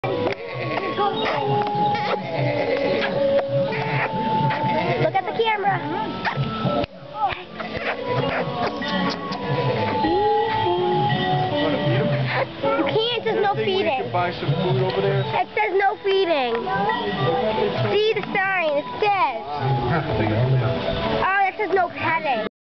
2008 - Storyland Vid by Amanda
Amanda Directs this warm and happy time at Storyland, NH.